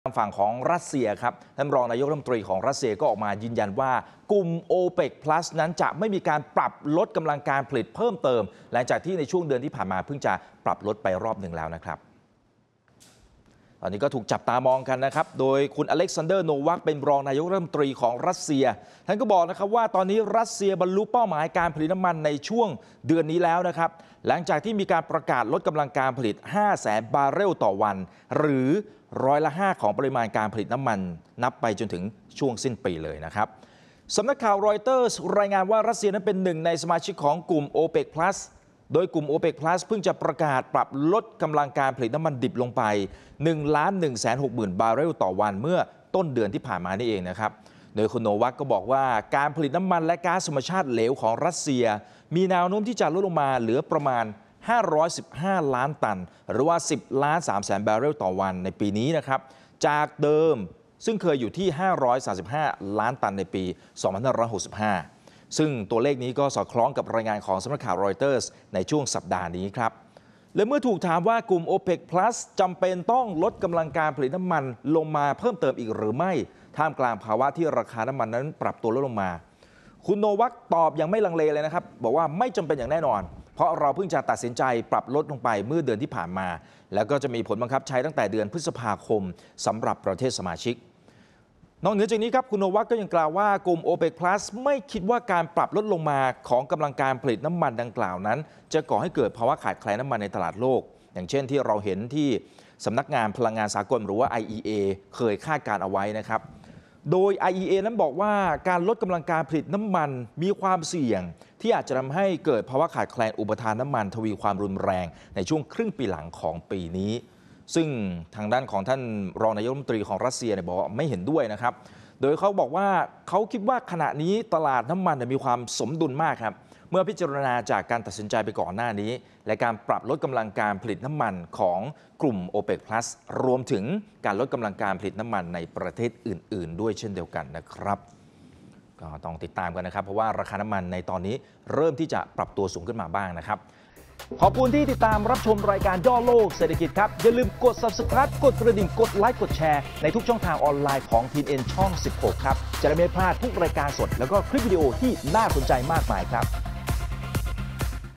โอเปกพลัสไม่จำเป็นลดกำลังการผลิตเพิ่ม | ย่อโลกเศรษฐกิจ 28 เม.ย.66
นายอเล็กซานเดอร์ โนวัค รองนายกรัฐมนตรีรัสเซีย เผย ขณะนี้กลุ่มประเทศผู้ผลิตน้ำมันโอเปกพลัส (OPEC+) ไม่เห็นถึงความจำเป็นในการปรับลดกำลังการผลิตน้ำมันดิบเพิ่มเติม แม้อุปสงค์ของจีนจะต่ำกว่าที่คาดการณ์ไว้ และโอเปกพลัสมักมีการปรับเปลี่ยนนโยบายด้านกำลังการผลิตน้ำมันเสมอก็ตาม